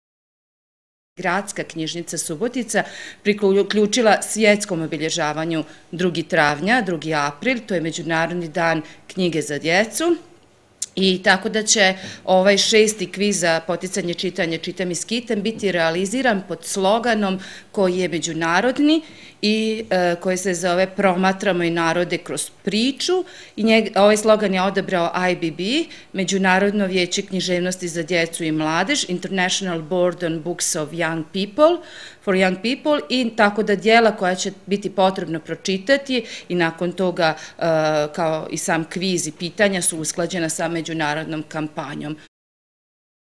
Kviz Gradske biblioteke "Čitam i skitam" počinje 1. februara
Šesti kviz za podsticanje čitanja pod nazivom „Čitam i skitam" u organizaciji Gradske biblioteke Subotica počinje u subotu 1. februara i trajaće do 31.marta. Kviz je namenjen  učenicima osnovnih škola u Subotici, a sprovodi se na srpskom, mađarskom i hrvatskom jeziku. Da bi deca mogla da učestvuju u kvizu, neophodno je da budu članovi Gradske biblioteke, a ukoliko to nisu, za vreme trajanja kviza mogu postati članovi ili obnoviti članstvo za vreme trajanja kviza, po promotivnoj ceni od 300 dinara.